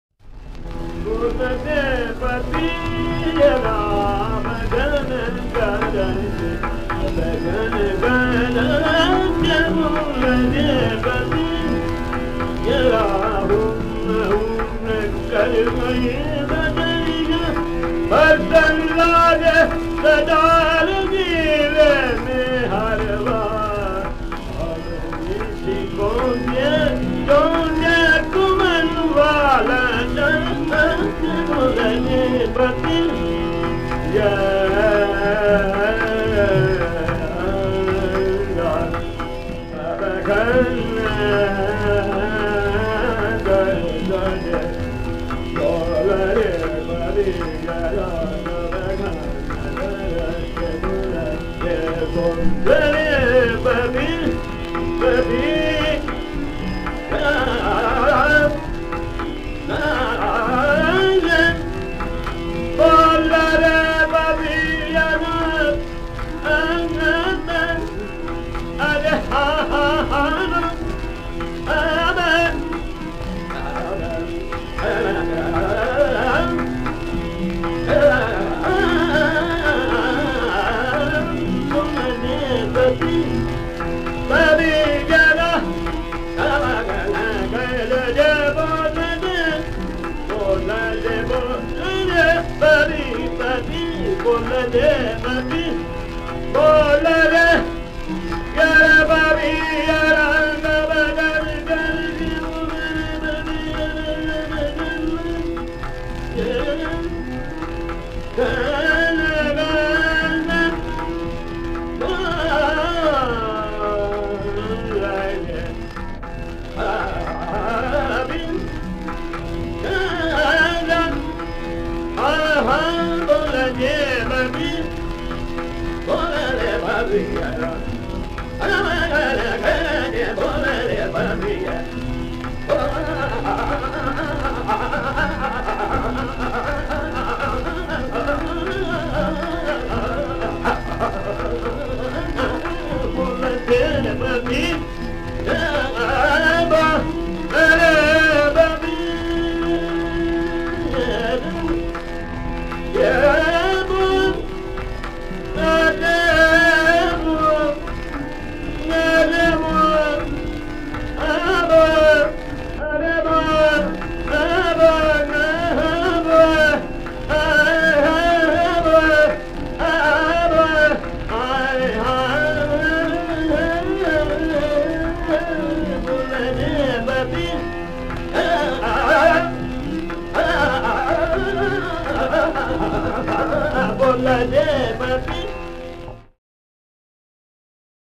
Ramkrishna Bua Vaze sings Raag "Malhar " - From Audio Archives of Lutfullah Khan
Ramkrishna Bua Vaze sings Raag "Malhar "
From Audio Archives of Lutfullah Khan